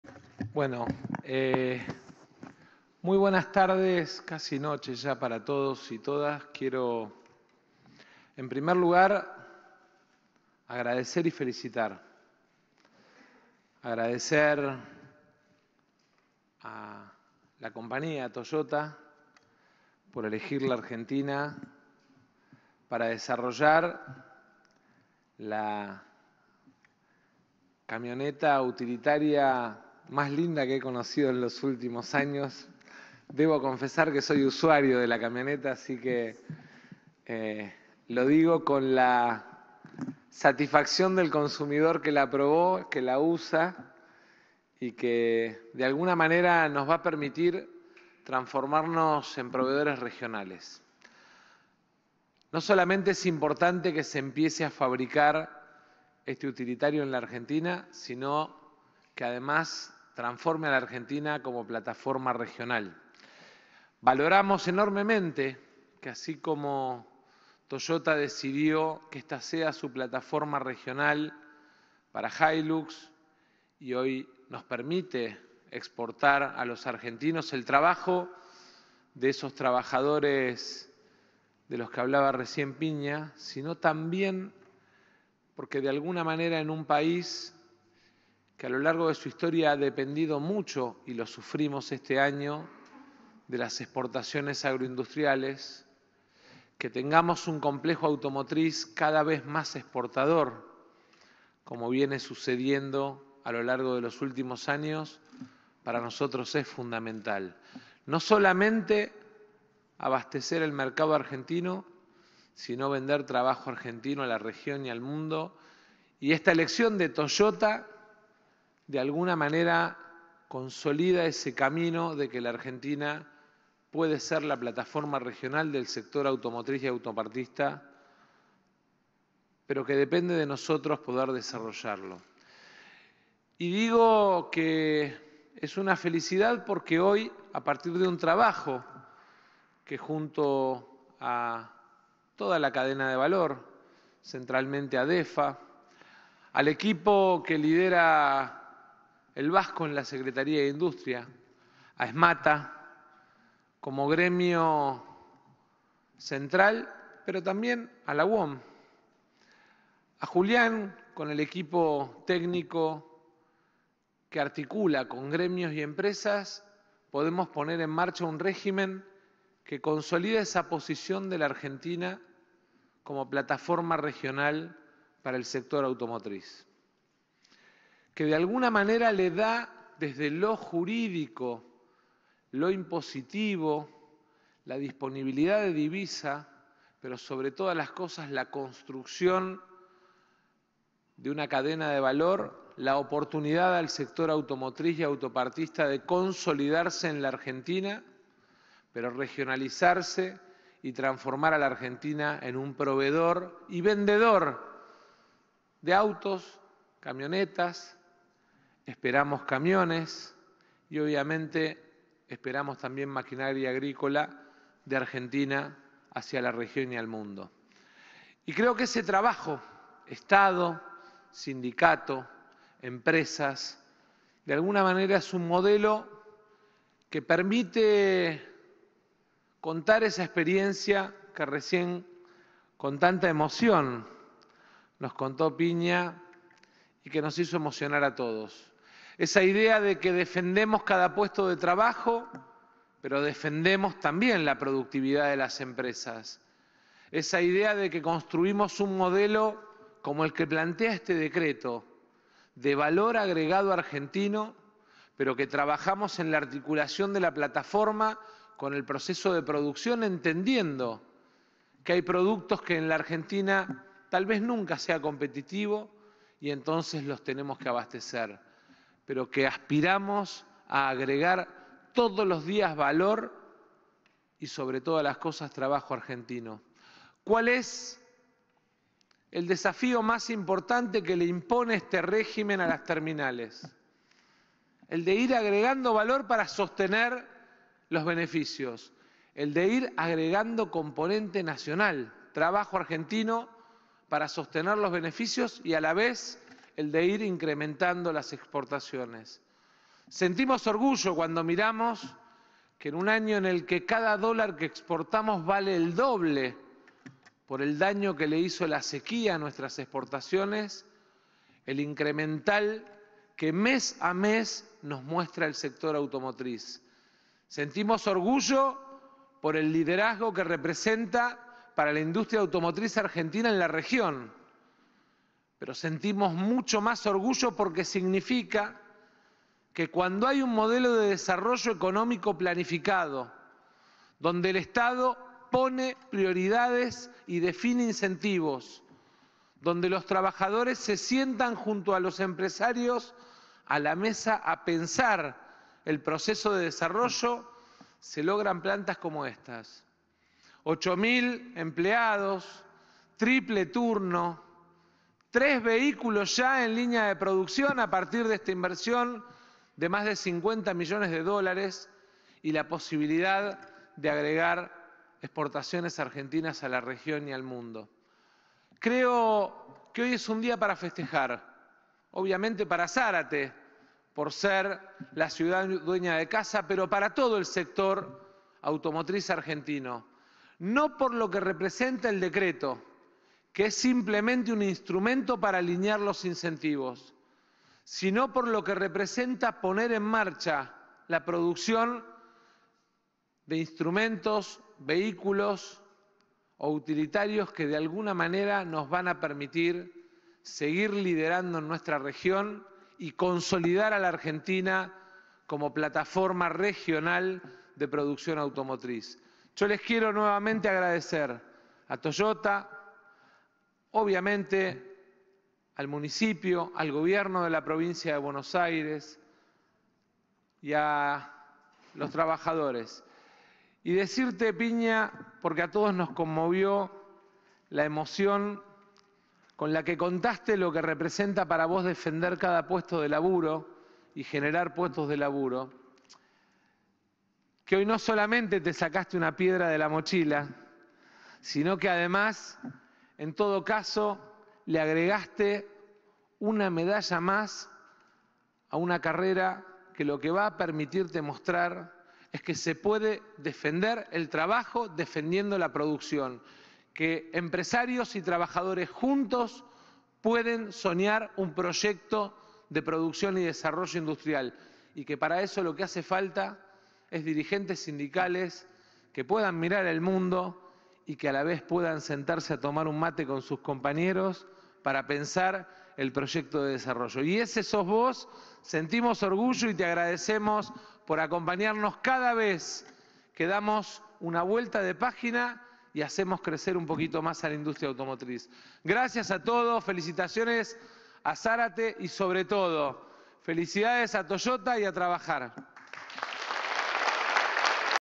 12-06-23 | IMPULSO A LA PRODUCCIÓN DE VEHÍCULOS | ▶️ DISCURSO
Con el objetivo de fomentar el desarrollo de una industria automotriz que exporte sus producciones y que utilice cada vez más componentes locales, lanzamos junto al Vasco de Mendiguren el Régimen de Impulso a la Producción de Vehículos para la Región en la fábrica de Toyota de Zárate.